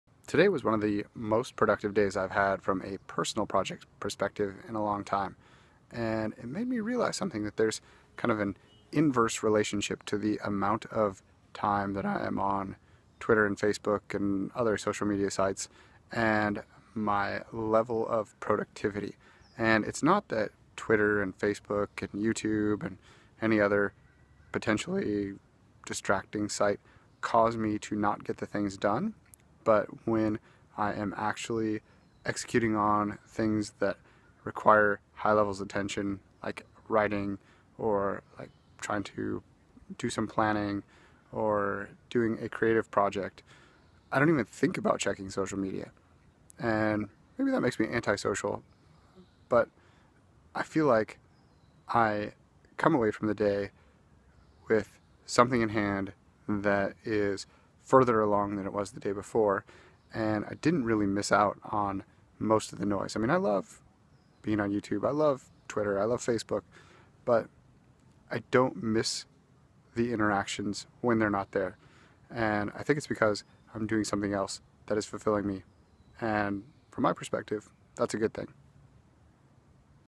The Inverse Relationship Between Productivity and Social Media
My most productive days also happen to be the days I'm least active on social media. My theory is that I enjoy the projects I'm working on enough that I don't use social media to fill in the gaps of time I normally have during days when my activities are less engaging.